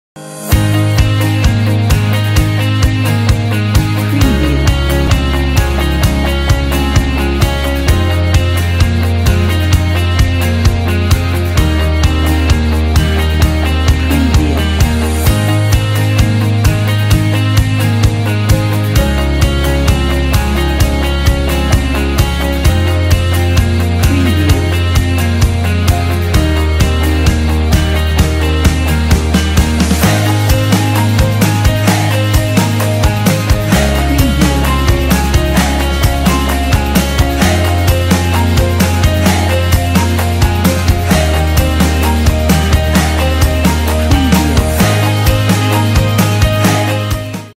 News Tickers Premiere Pro Templates
The News Tickers is a handy and easy tool for creating a creeping line (running text). This is perfect for news broadcasts, updates, and more projects that refer to current events. Use for any formats and themes of video: news, sports, advertising, video blog, and others. Special made for 2nd wave of COVID-19 situation in the world.